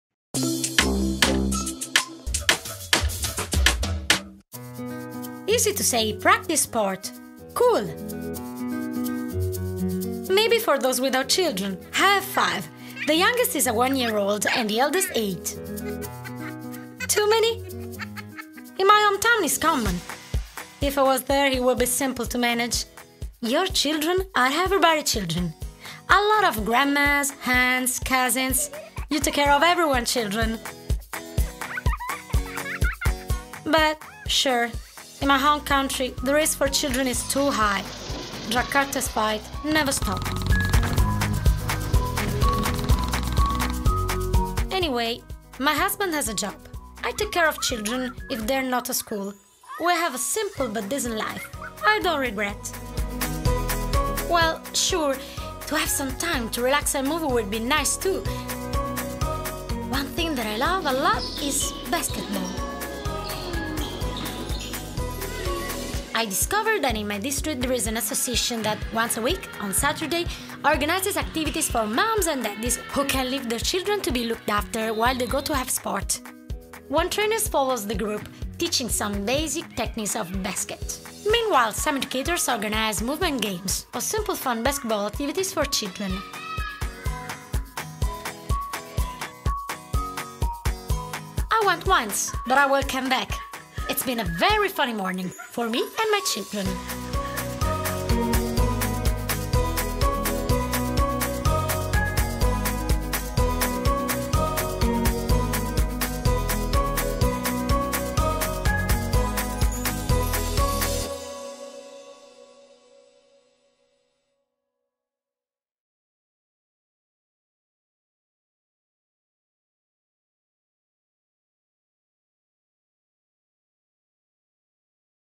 Sport and women's rights with the SPIN WOMEN project: Paula and the time
For migrant and refugee women, there are still many barriers to sports. Seven short videos, characterized by the colors of the rainbow, tell the problems or positivity encountered by migrant women in practicing sport in the countries where they were welcomed. The videos are freely inspired by the different testimonies collected during the research conducted by the German partner Camino. Project co-financed by the European Commission (Erasmus +), of which Uisp is a partner together with six European organizations: VIDC (Austria - lead partner), Camino (Germany), FAI (Ireland), Likkukaa (Finland), Mahatma Ghandhi for Human Right Organization ( Hungary), Sindiacato dos Jogadores (Portugal).